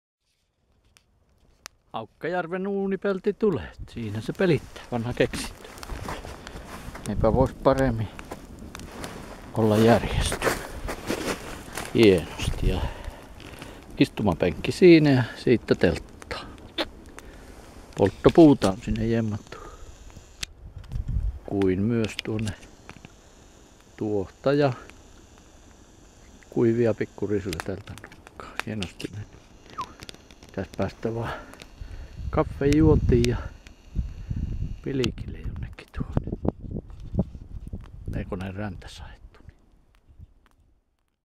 Fire on Ice. Saves Wood, Nerves And the Nature, näätäkoistinen, Kemi
Campfire on an owenplate. Easy to set up, makes no damage to the ground. Suitable for every season. Under the fire you can keep your food warm. Easy to carry on backbag.